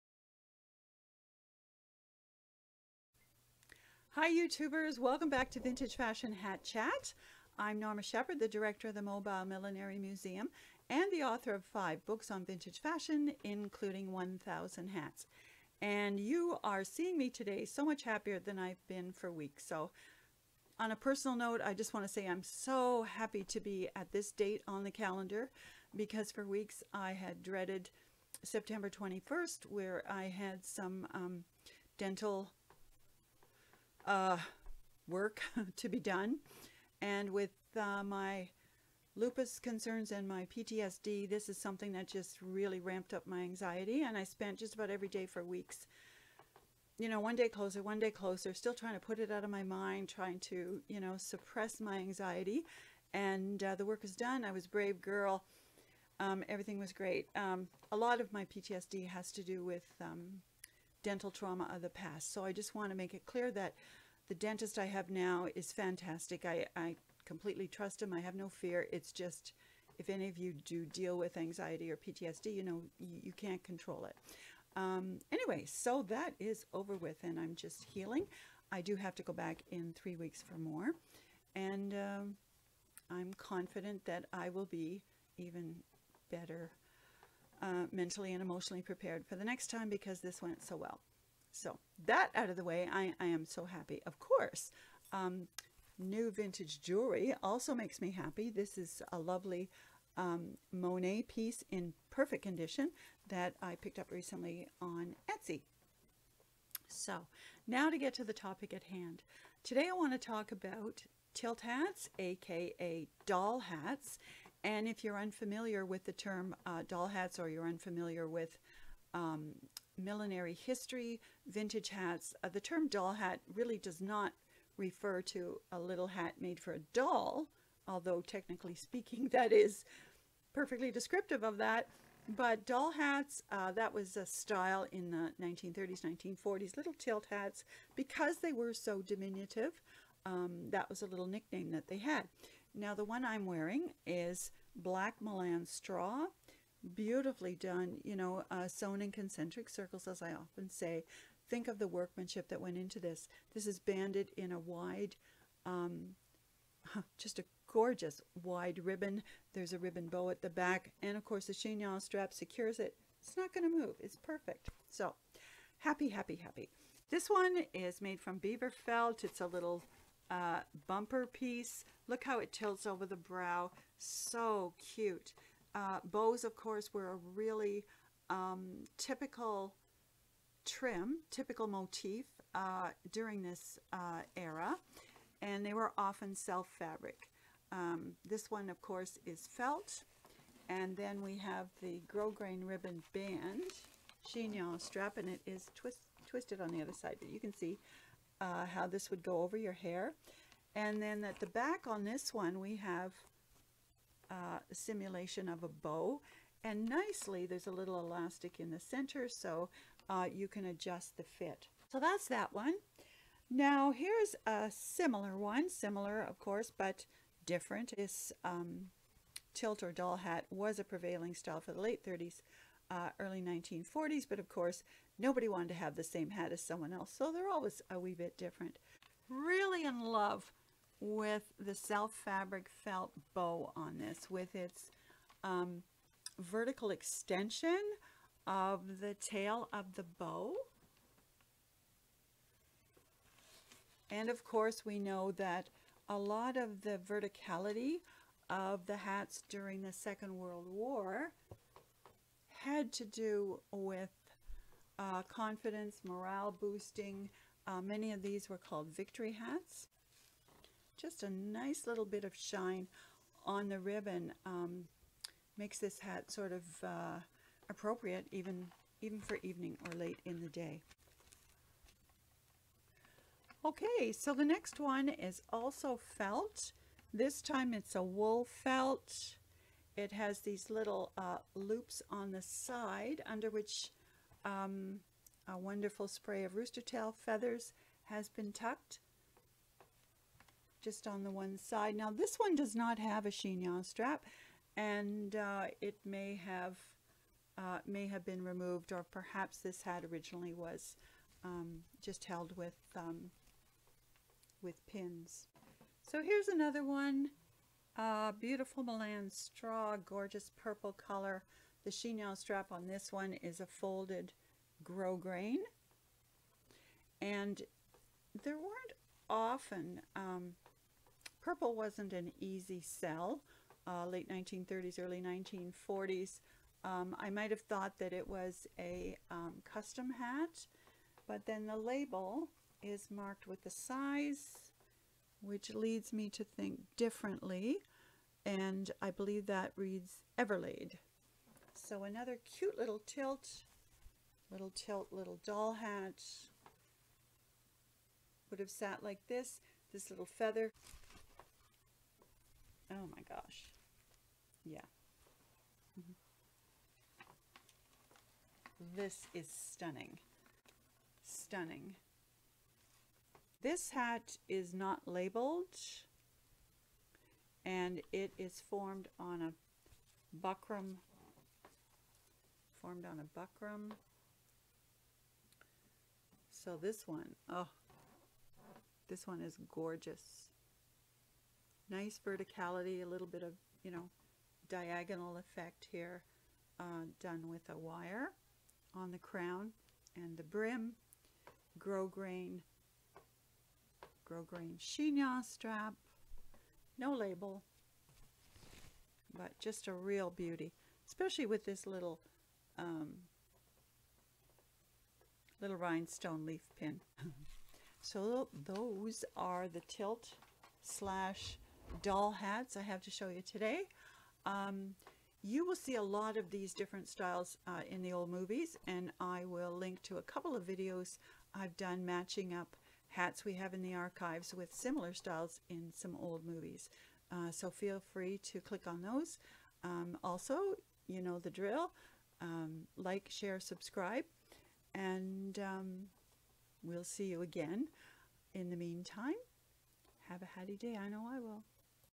Hat Happy, Jewelry Joy, & PTSD/ Tilt/Doll hats from my Montreal vintage hat haul
💓💓💓Links💓💓💓